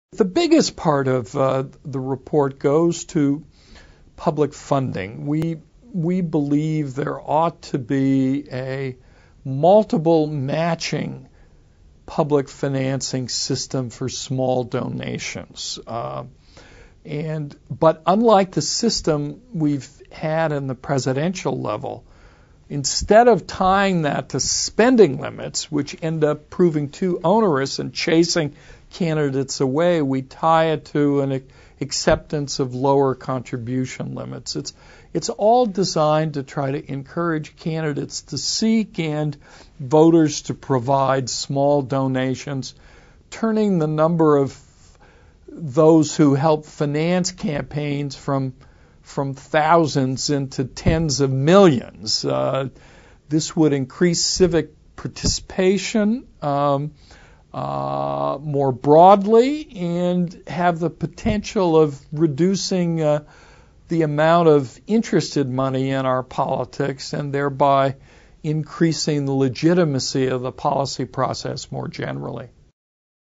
Expanding Participation
The report calls for expanding civic participation with public funding, bringing millions of people into the process while decreasing the need for large donations from lobbyists, Mann says.